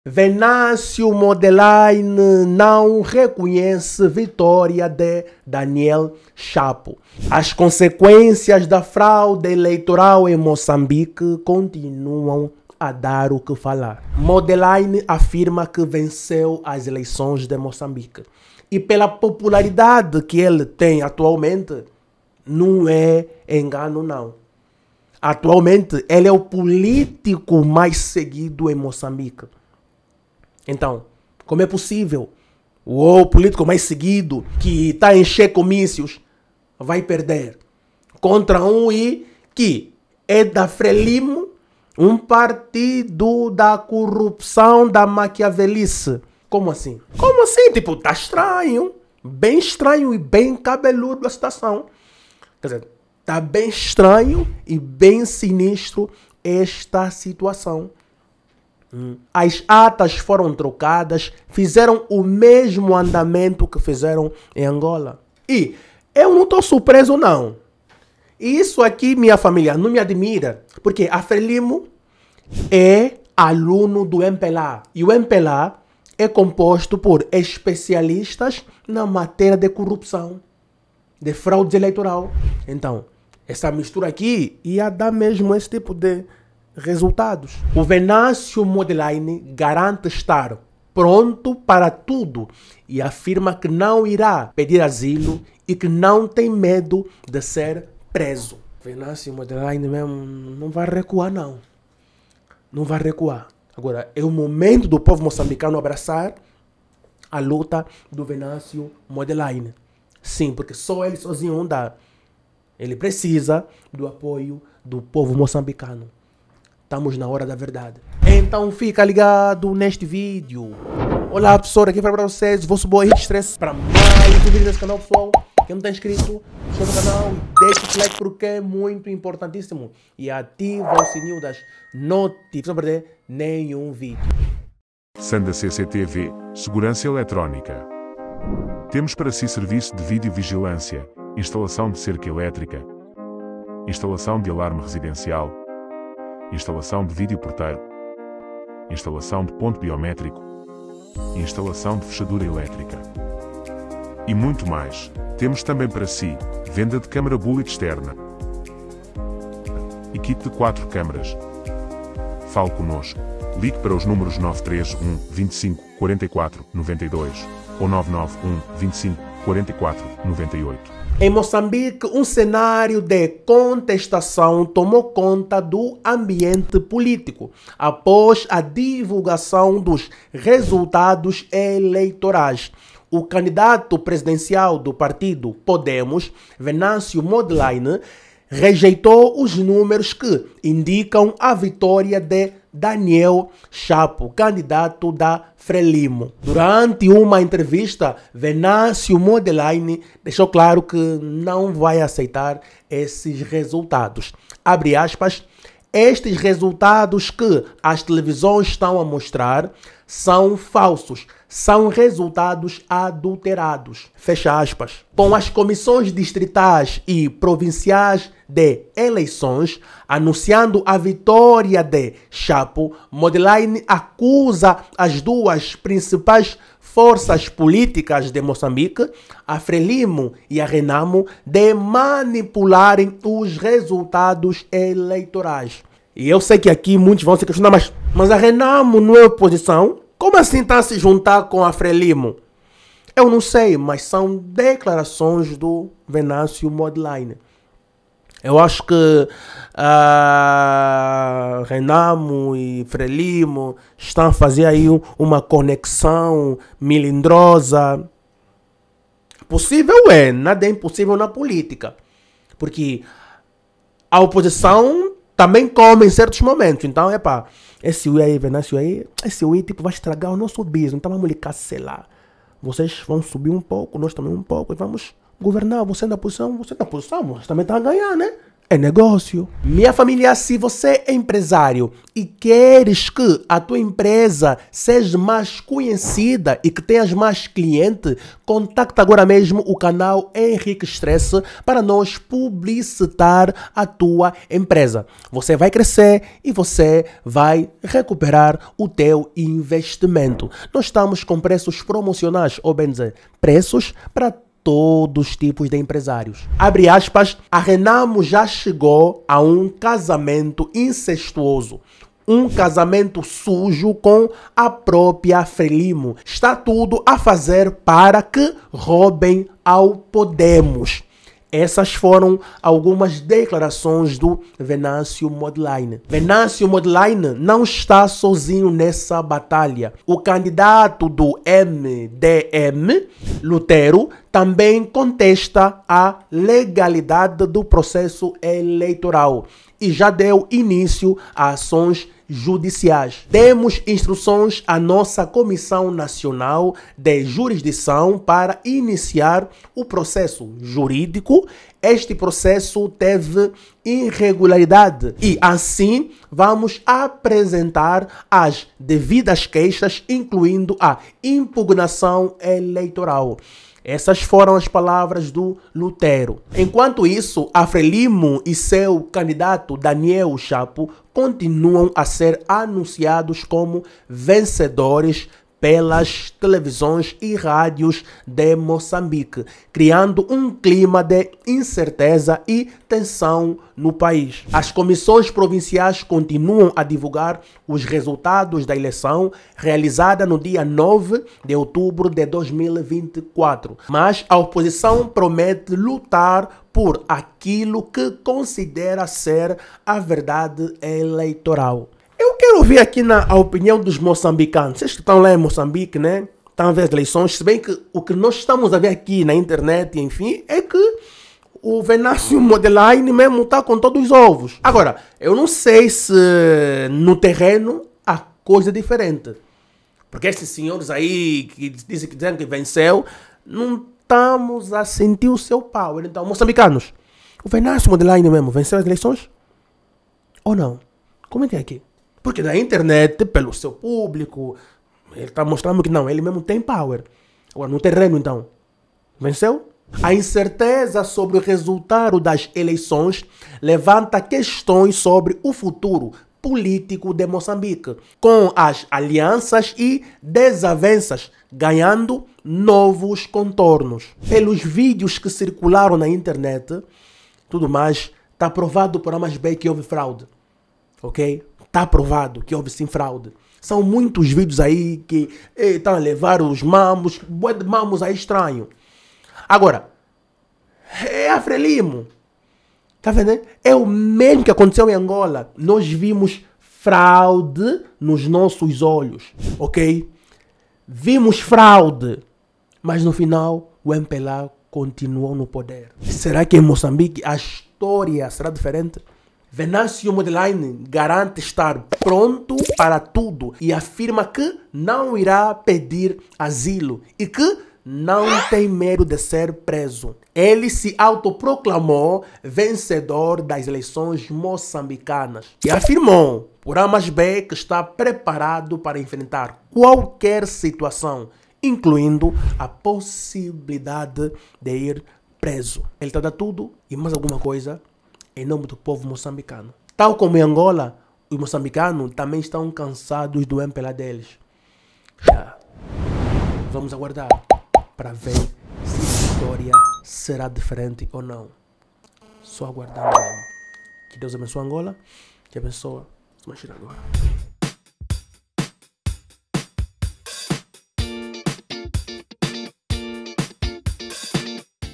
Mondlane garante estar pronto para tudo e afirma que não irá pedir asilo e que não tem medo
Venâncio Mondlane garante estar pronto para tudo e afirma que não irá pedir asilo e que não tem medo de ser preso

Contacto Profissional
Banco Sol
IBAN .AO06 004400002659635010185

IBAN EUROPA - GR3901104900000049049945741
faca a sua (doação) para ajudar o canal